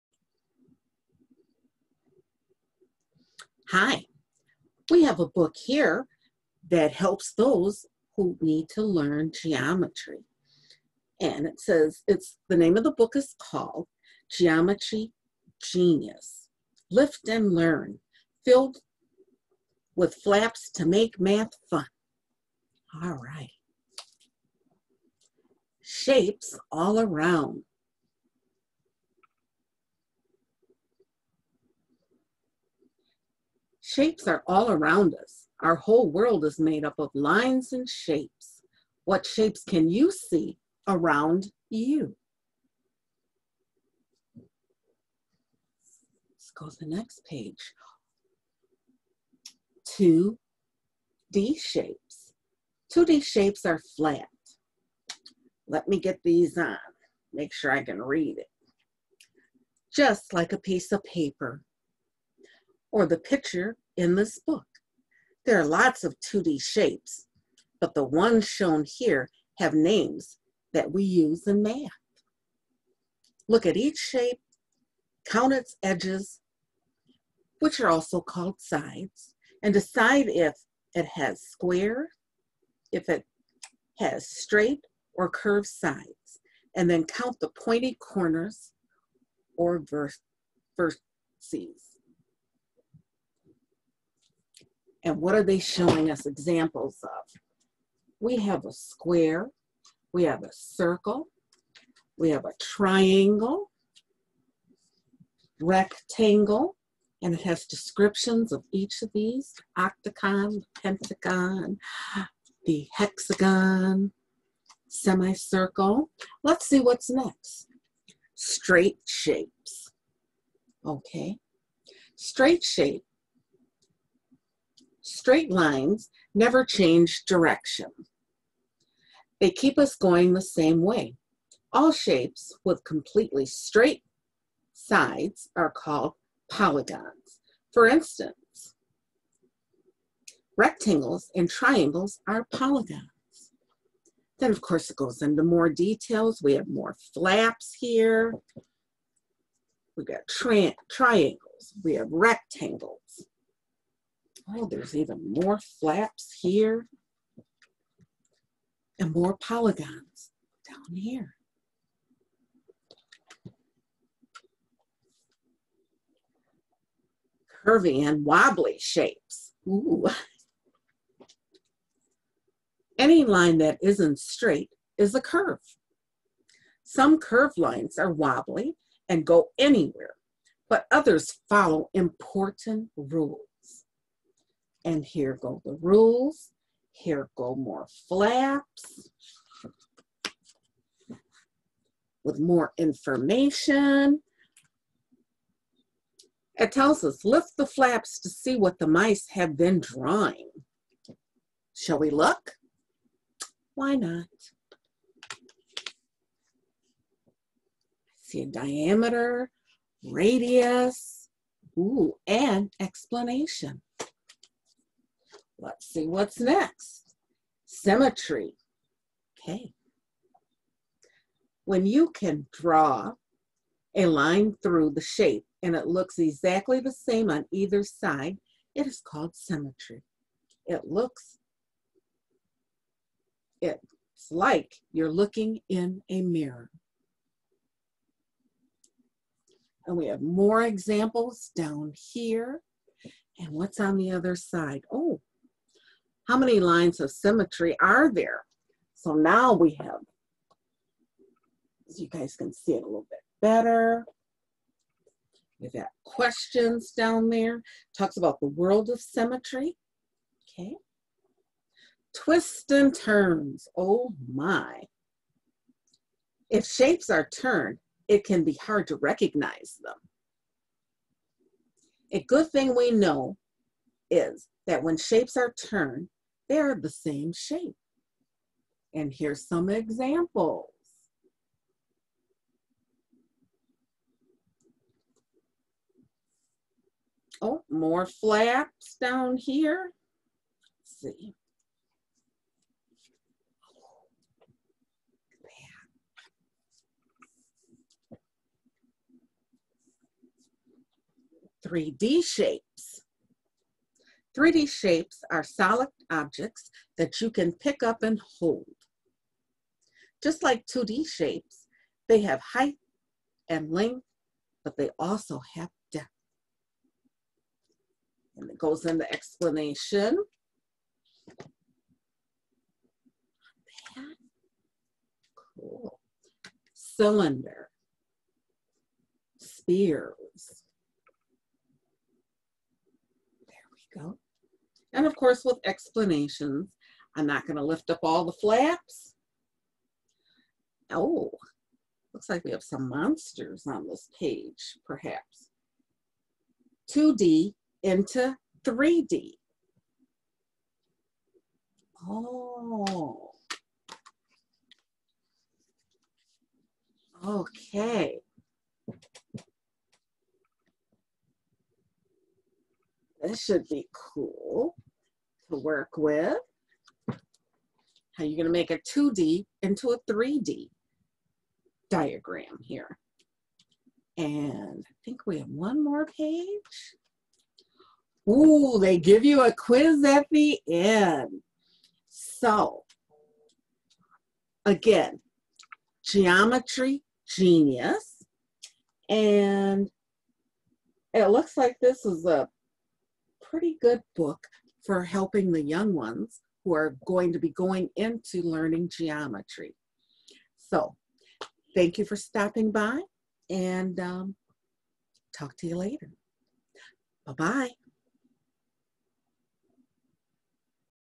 Geometry Genius
Geometry Genius - By DK

An interactive guide to shapes for 5 to 8-year-olds, this bright and bold lift-the-flap activity book helps children understand the properties of 2-D and 3-D shapes. Shapes are an important topic for early learners, and this visually appealing book will make it a lot of fun, too!

Features fun geometric characters, like Fox and Lion, and lift-the-flap activities that help kids relate shapes to everyday life. Characters pose key questions, such as “What’s special about a sphere?,” “What is an equilateral triangle?,” and “How many lines of symmetry does a hexagon have?” Children can then lift the flaps and find the answers. An interactive pop-up will also bring learning to life by encouraging kids to spot different shapes within the scene.

Helps kids identify and describe 2-D and 3-D shapes, compare and contrast features of regular and irregular shapes, discuss the size and orientation of shapes, understand nets, identify and count lines of symmetry, and more! It gets kids thinking about shapes in their world and not just on the pages of a math book.

 Quiz questions and fun activities are found sprinkled throughout the book, encouraging kids to lift the flaps and find out more. Learning shapes is a highly visual topic, and this book tackles the subject in a visually appealing, fully interactive way.

 Hardcover lift-the-flap board book, 19 pages.
5 years+